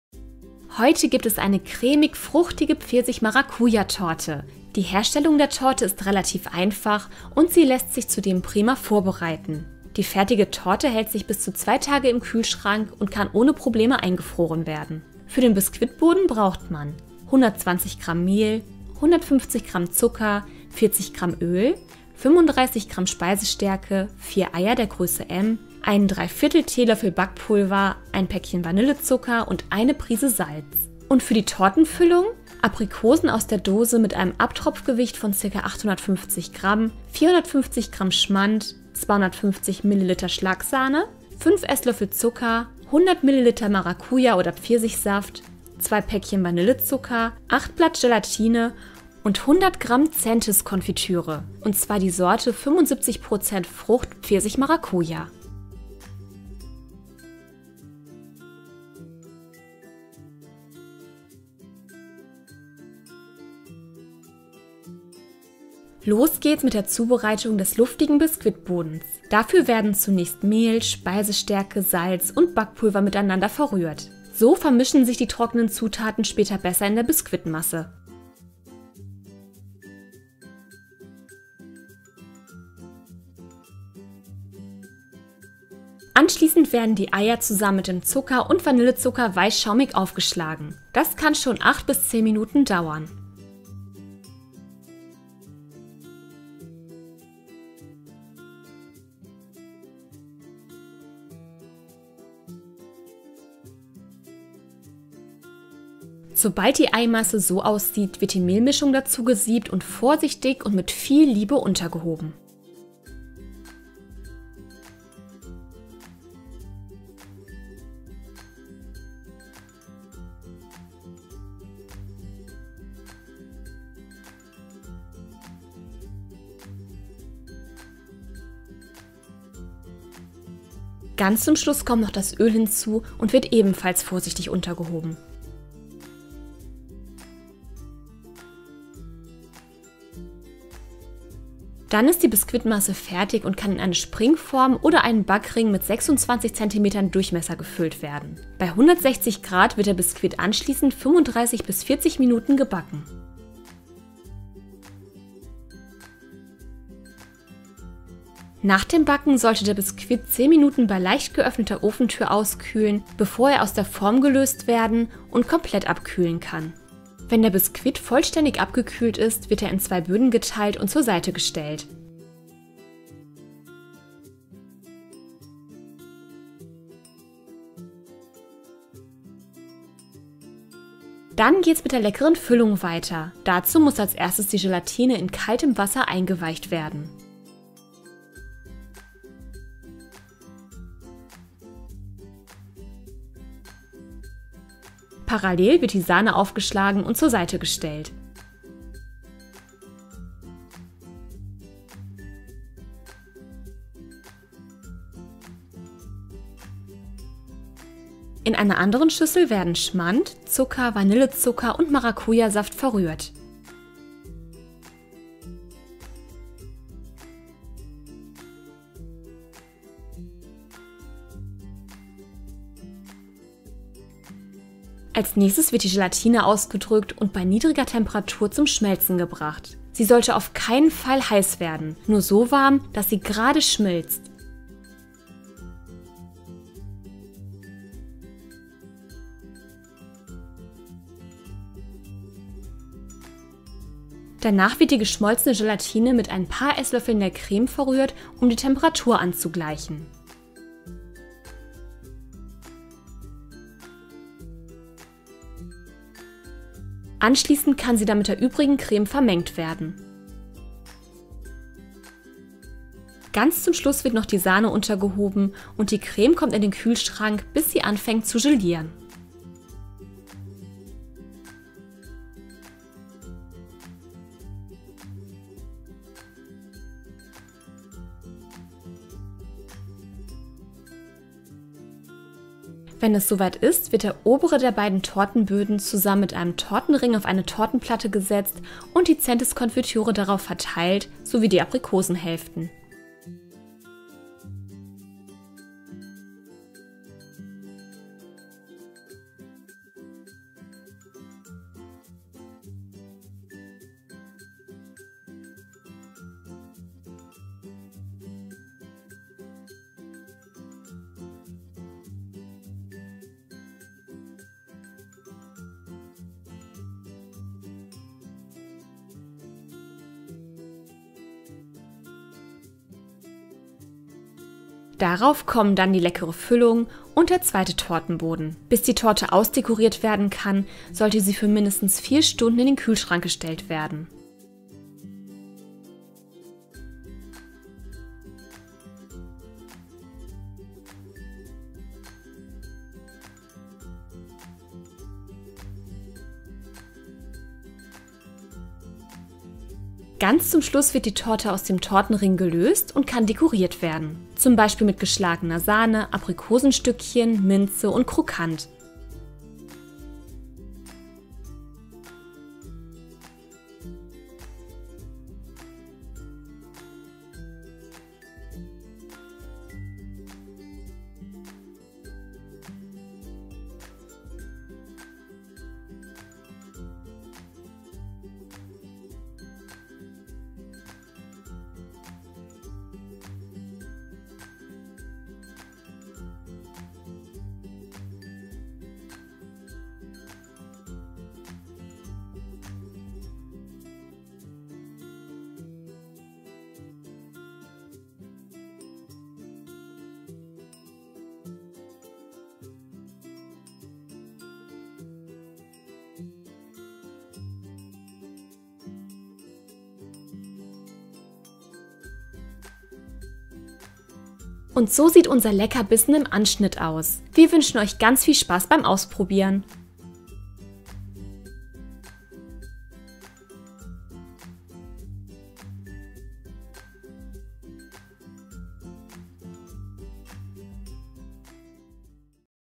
Exotisch fruchtige Sahne-Torte
Himmlisch cremig, exotisch fruchtig und herrlich locker. So begeistert diese köstliche Torte zu jeder Gelegenheit. Unsere feine Konfitüre 75 % Frucht Pfirsich-Maracuja gibt der Tortenkreation das besondere Etwas. Mit diesem Rezeptvideo von Kochkino könnt Ihr den sahnig fruchtigen Tortentraum ganz einfach nachbacken. Wir wünschen Euch gutes Gelingen und einen köstlichen Genuss beim Cremenaschen und Torteschlemmen.